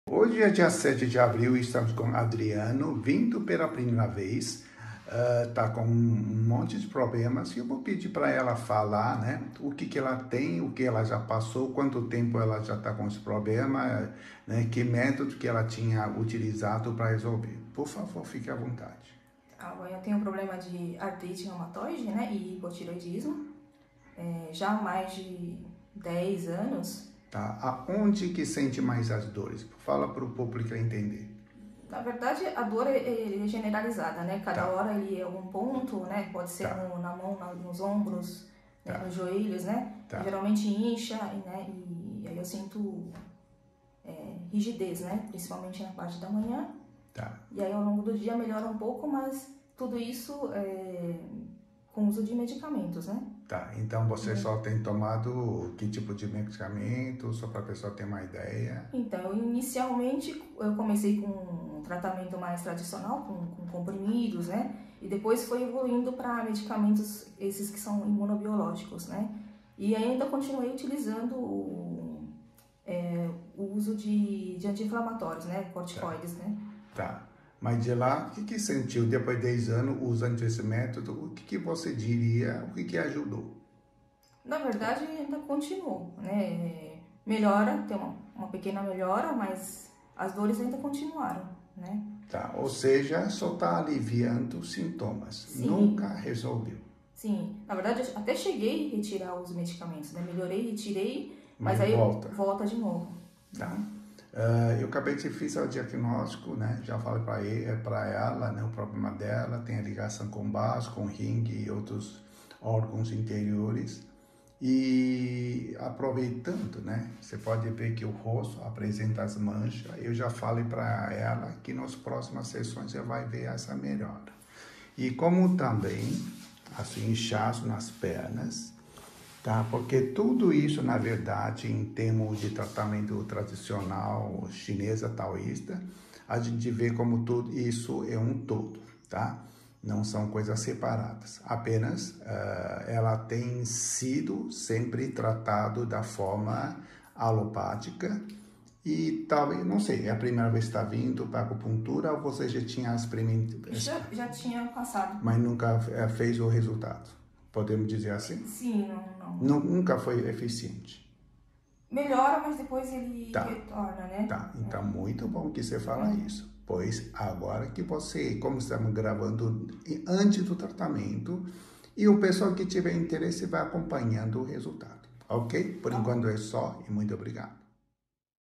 1ª sessão: Artrite reumatóide, hipotireodismo, dor no ombro, dor lombar, manchas no rosto e etc.
07/04/2023: Adriana antes da 1ª  sessão contou seu historico de problemas de saúde conforme descrito acima e o Prof. Tsai fez o diagnóstico para que possa acompanhar o resultado.

Link onde pode ara pesquisar seus sintomas em mais de 400 depoimentos feitos pelo pacientes: